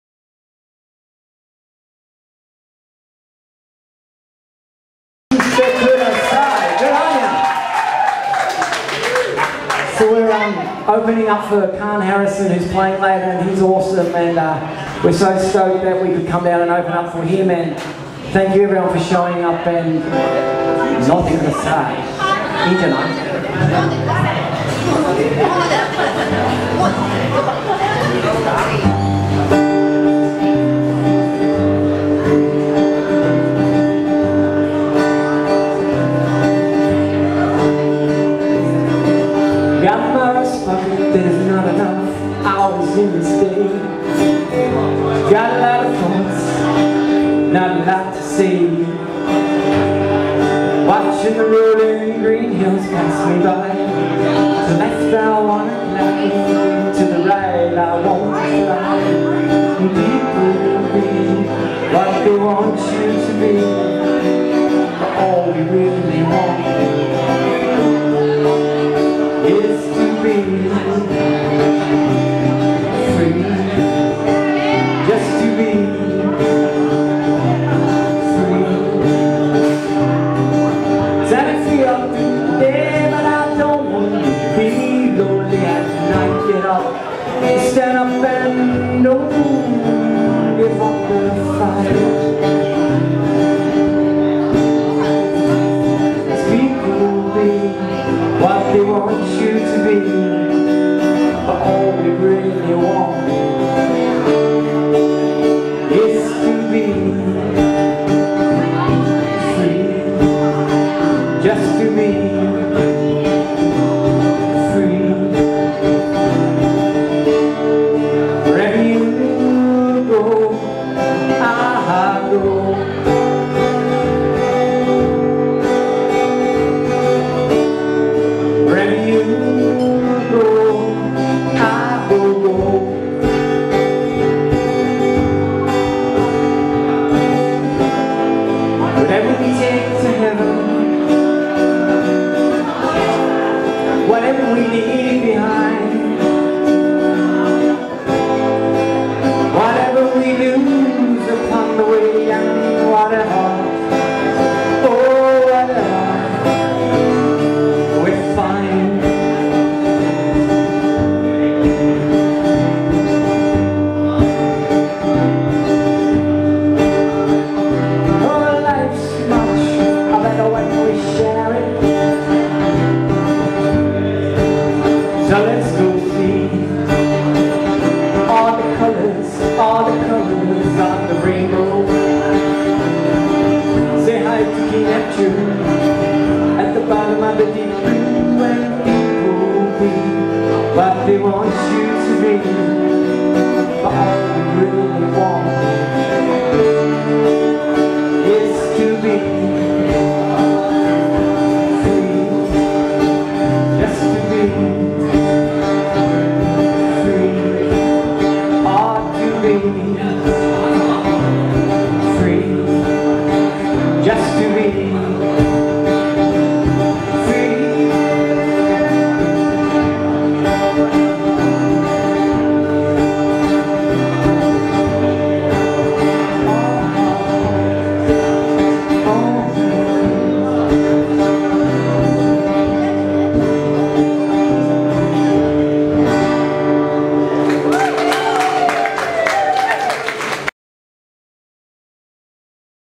MATTHEW PITTS FREE 2016
MATTHEW PITTS　Acoustic live music on Quiksilver Boardriders / Quiksilver Cafe Bar 28 Cooly, Coolangatta Gold Coast 18/Mar/2016